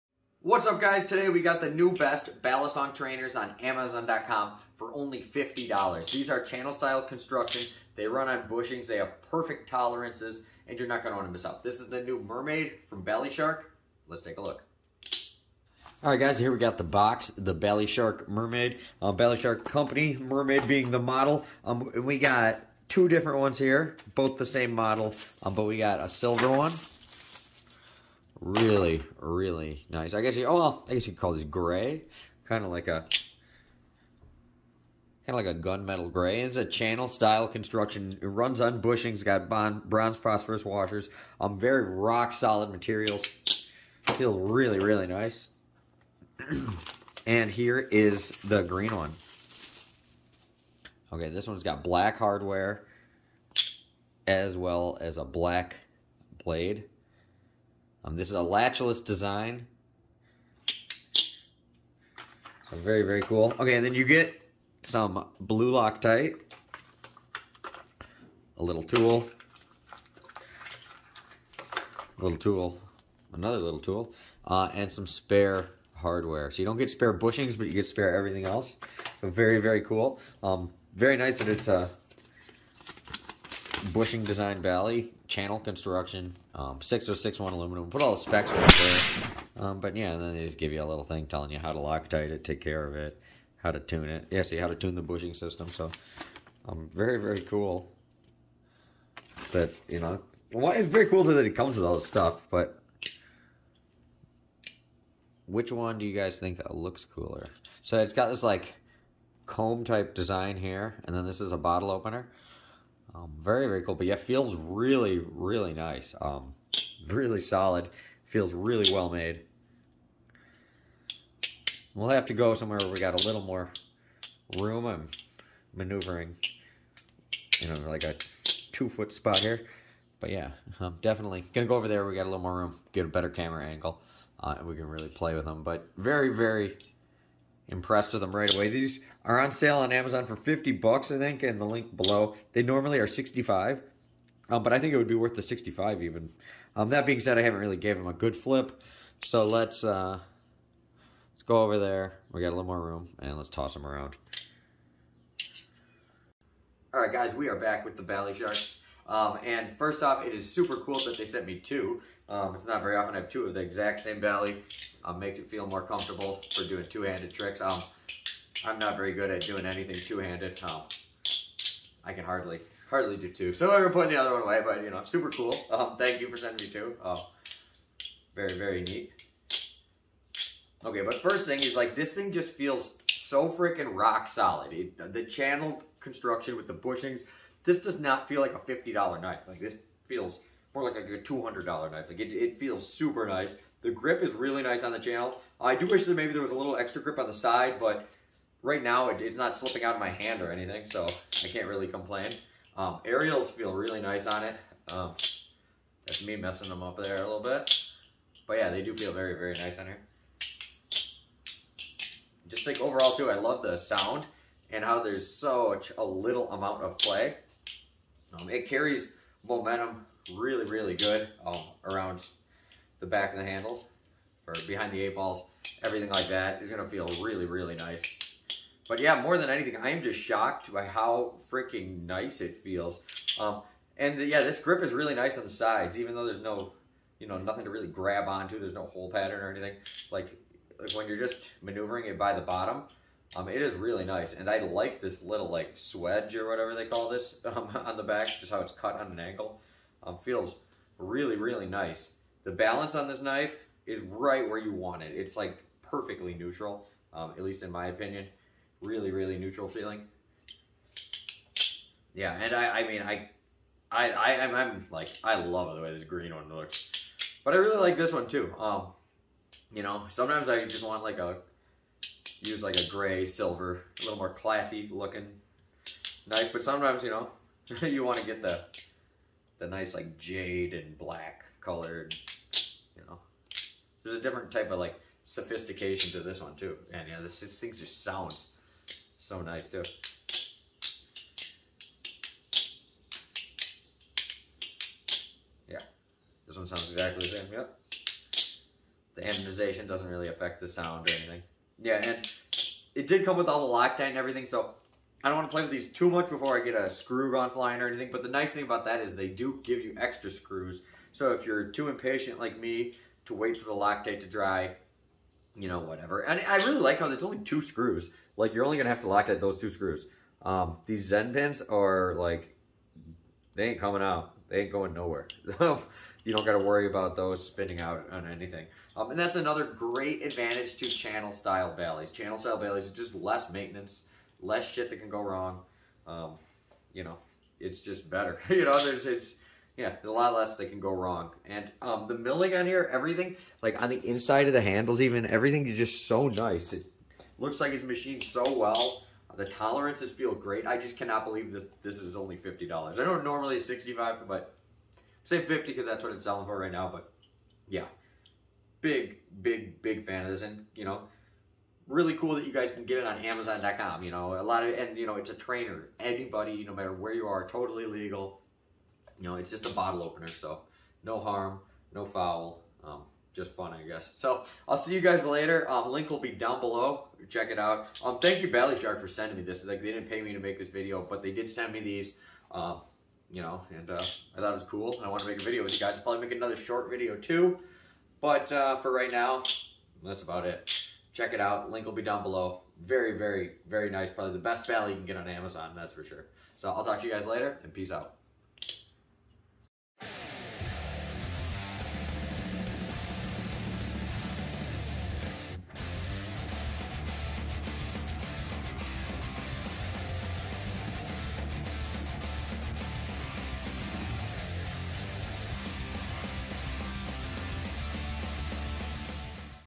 BaliShark Mermaid Butterfly Knife Trainer. Unboxing and Review. Best $50 balisong on amazon
Whats up guys today I got this awesome Balishark Mermaid channel handle bushing system balisong trainer off amazon.com This butterfly knife cost only $50 on amazon and it is worth every penny.